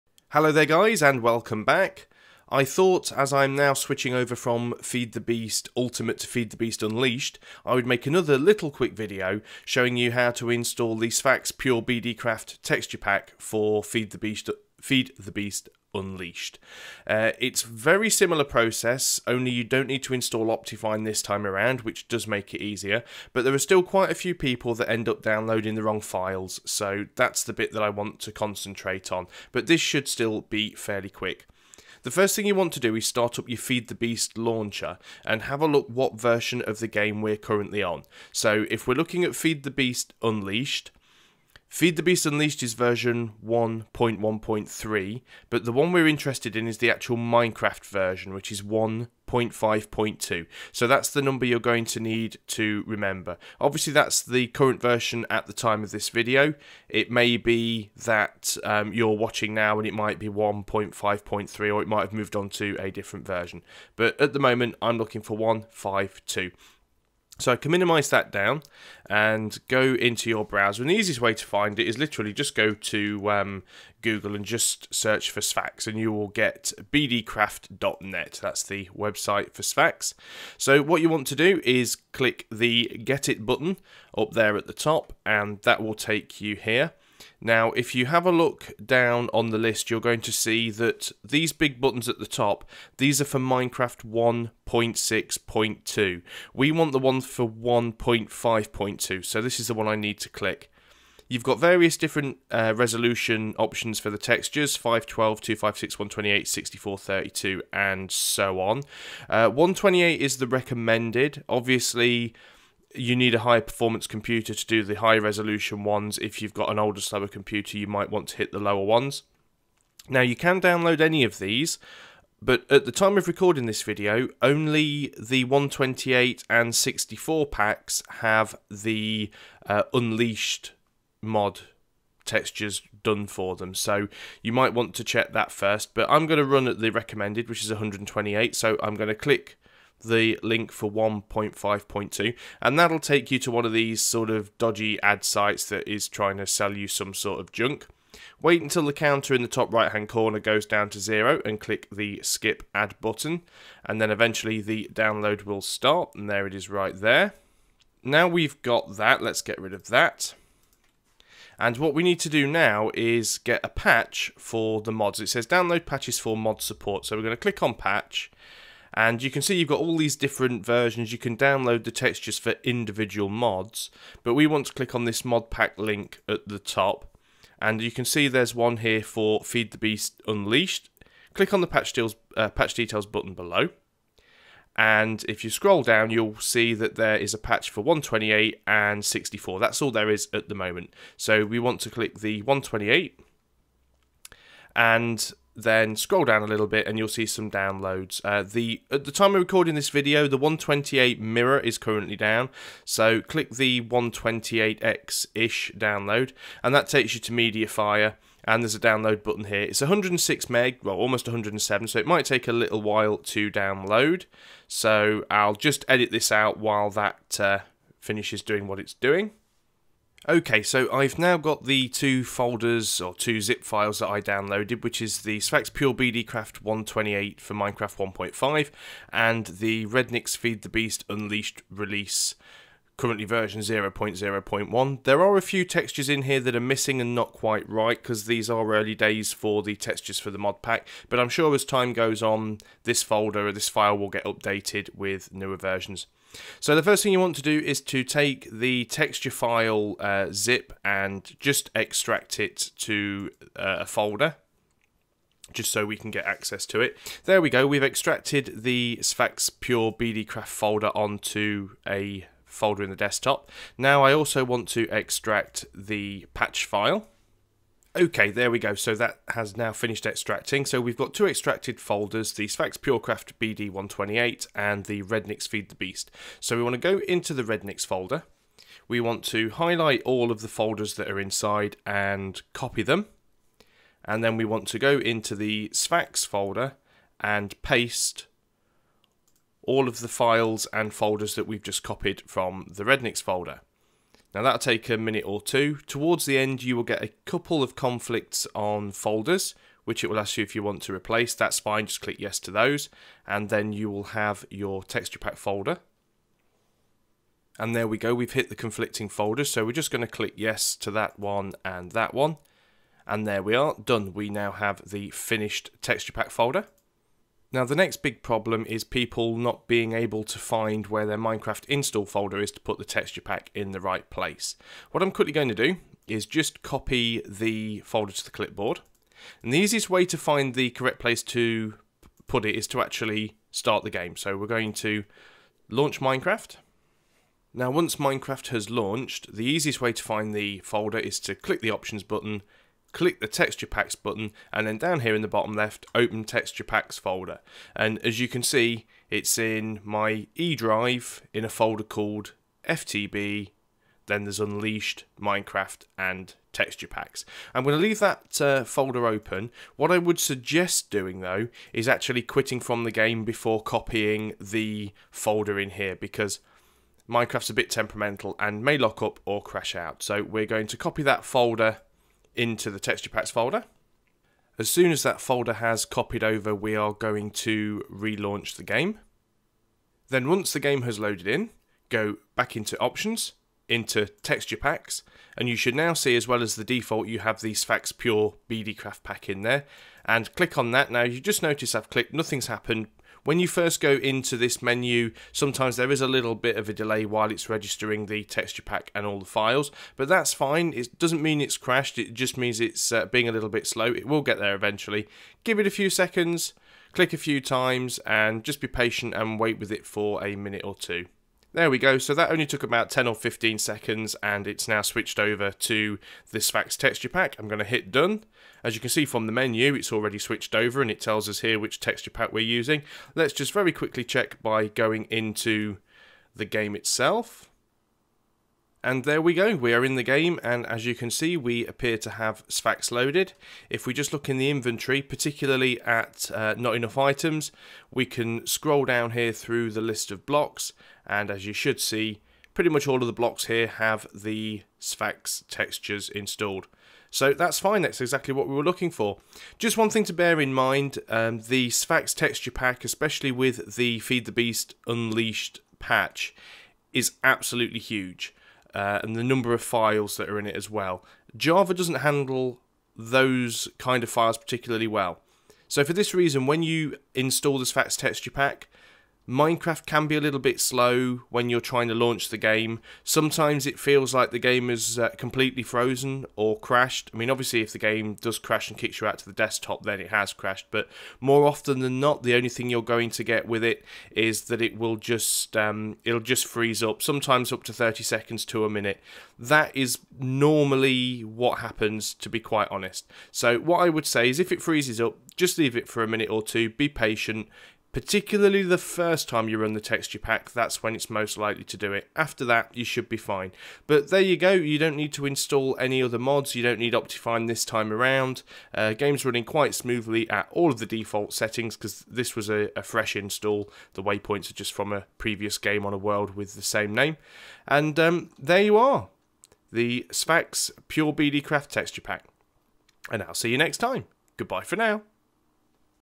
How To Install Sphax PureBDcraft Texture Pack in FTB Unleashed
These instructions are for installing Sphax in FTB Unleashed.

In this video I explain how to download and install the Sphax PureBDcraft texture pack for FTB (Feed the Beast) Unleashed.